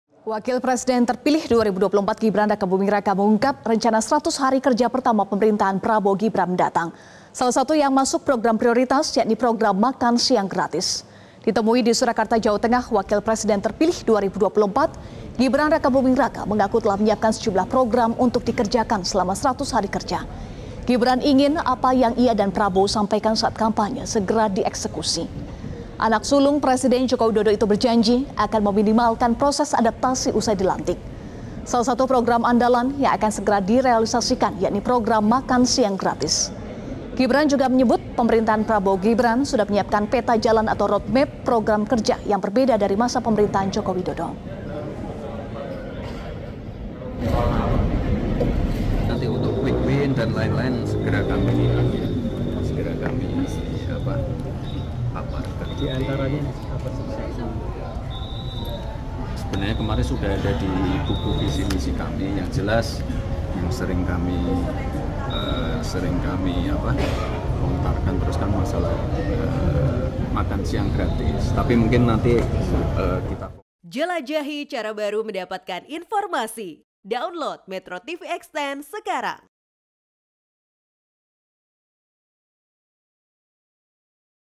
Gibran: 100 Hari Kerja, Prioritas "Makan Gratis"
MetroTV, Wakil Presiden Terpilih Gibran Rakabuming Raka mengungkap rencana 100 Hari Kerja Prioritas pemerintahan Prabowo-Gibran Mendatang. Salah satunya ia menyebut program Makan siang gratis.